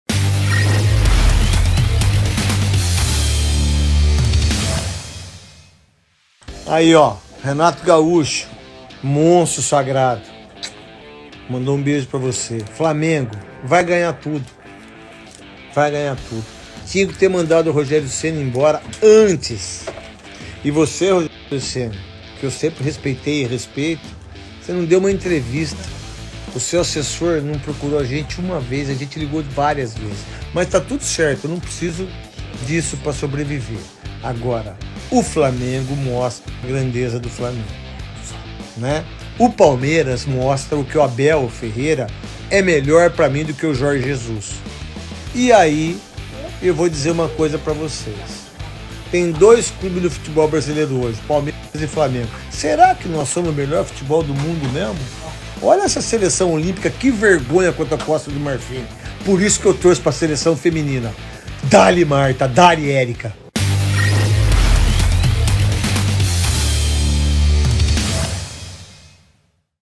CRAQUE NETO COMENTA GOLEADA NO JOGO ENTRE FLAMENGO E SÃO PAULO | PLANTÃO DO CRAQUE NETO #7
Fala, garotinho! Os outros times que se cuidem, o Flamengo com o Renato Gaúcho vai arrebentar. E eu sempre falei, o Renato é monstro sagrado! Só o Palmeiras consegue competir.

Se liga na programação:
Às terças rola o “Neto Responde”